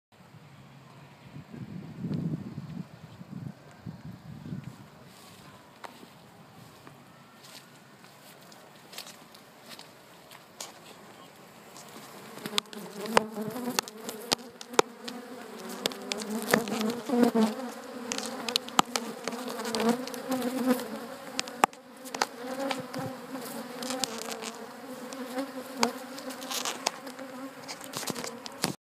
Live Africanized Killer Bee Attack! Unprovoked!!!!!
Luckily, I was wearing my protective bee suit when I filmed this. As you watch this video, everytime you hear a clicking sound it's the sound of a bee ricocheting off my bee suit, mostly around my head and torso. Thank goodness for the hat design. Each click you hear is a bee sting that was prevented by the protective suit. This is a classic attack of africanized killer bees. Usually, bees do not attack like this. I've stood 5 inches away from a hive without them attacking like that. By the behavior of these bees, and the nature of their killer instinct attack, I can safely say that this was an africanized killer bee hive. By order of the State, we have to take down the hive permanently. An attack like this, without the proper protection will kill an adult human before they can get to safety. I have to admit, even though I was protected by the suit, my heart was racing the whole time, because the suit is "sting resistant", not "sting proof".  I have been stung through the suit before, luckily this was NOT one of those times. I walked away bee sting free.  ;)